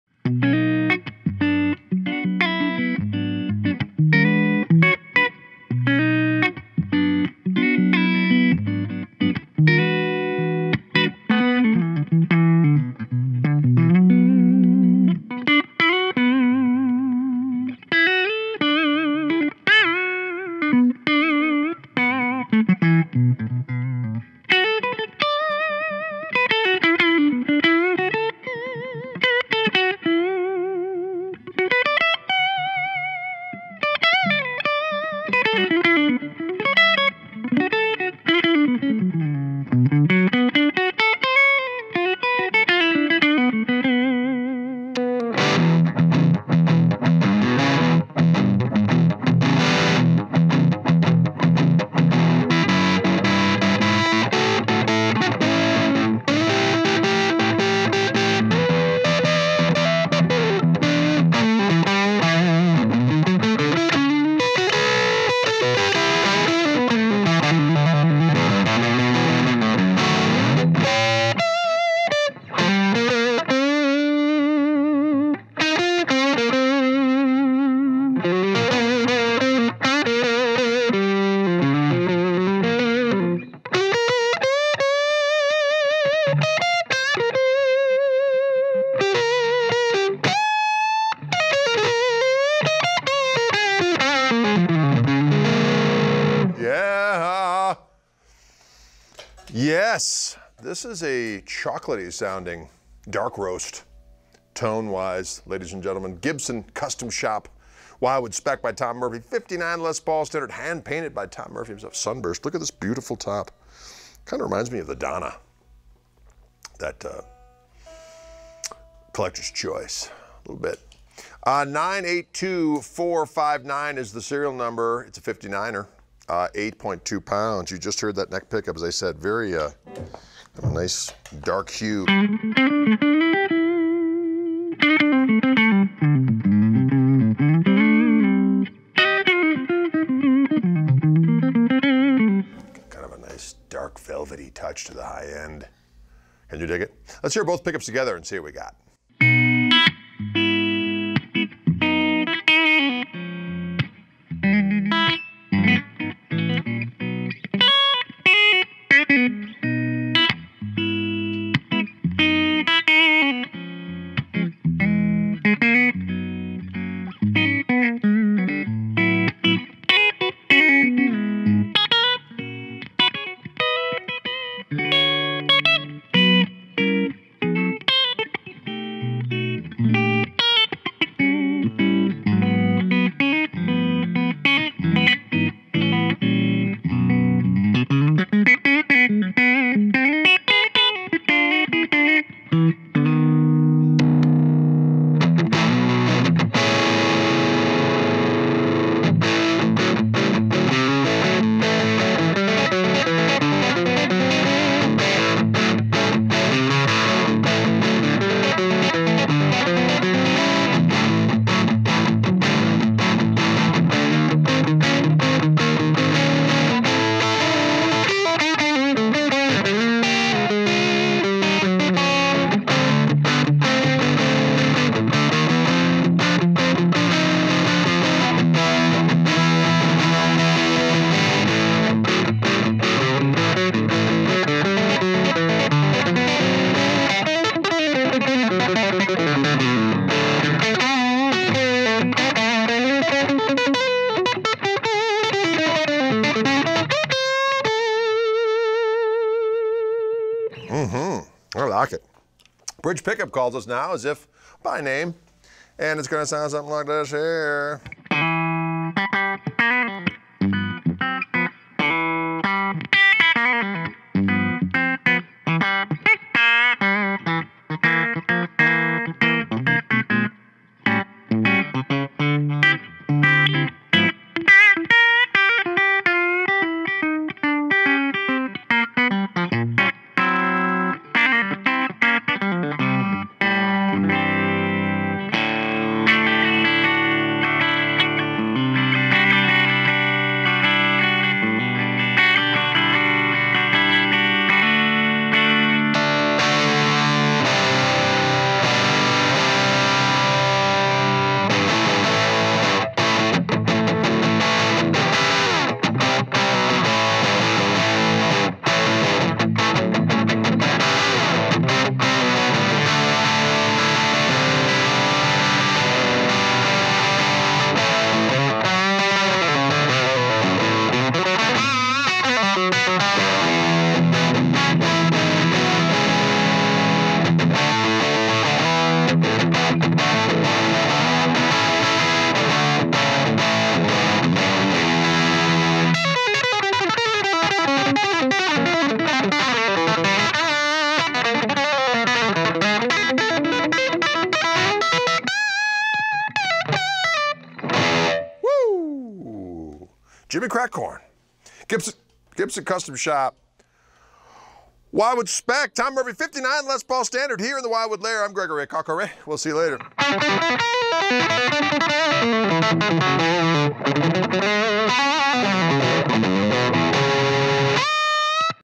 Gibson Custom Shop Wildwood Spec by Tom Murphy 1959 Les Paul Standard • SN: 982459
The Wildwood Spec by Tom Murphy Les Paul Standards are so much more than meets the eye. We began by hand-selecting premium maple tops and Indian Rosewood fingerboards, and we carefully matched them to give each instrument its own unique visual aesthetic. Then, Tom did the rest with his wizard’s touch. He painted each and every one of these instruments completely by hand, and they also feature his ultra-accurate top carve, hand-stamped hangtags, all-hide glue construction, and reinforced metal jack plates. To complete the package, each of these Les Pauls features our proprietary Wildwood Spec Custombucker pickups, which we believe offer the ultimate vintage PAF sound. These phenomenal instruments have been personally crafted to look and sound as authentic as humanly possible, and we believe this dedication to the details is why they capture the mojo of our favorite vintage Les Pauls with such jaw-dropping precision.

Model Wildwood Spec by Tom Murphy 1959 Les Paul Standard
Aging Murphy-Aged
Finish Color Hand-Painted Tom Murphy Sunburst
Top Wood Hand-selected 2-Piece Figured Maple attached with Hot Hide Glue
Body Wood Lightweight Solid Mahogany
Neck Wood 1-Piece Mahogany Neck with Long Neck Tenon
Neck Shape '50s Rounded Tom Murphy "C"
Neck Dimensions .880 1st - .990 12th
Fingerboard Rosewood attached with Hot Hide Glue
Fingerboard Radius 12"
Inlays Cellulose Trapezoids
Frets 22
Binding Single-Ply Cream on Top and Hand-Filed Rolled Fingerboard Binding
Pickups 2 Custom Buckers wound to Wildwood Spec
Controls 2 Volume, 2 Tone and 3-way Toggleswitch
Hardware Nickel
Bridge ABR-1
Tailpiece Lightweight Aluminum Stopbar
Tuners Kluson "Deluxe"
Unique Features Vintage Accurate Plastic Parts
Case Gibson Custom Hardshell
COA Yes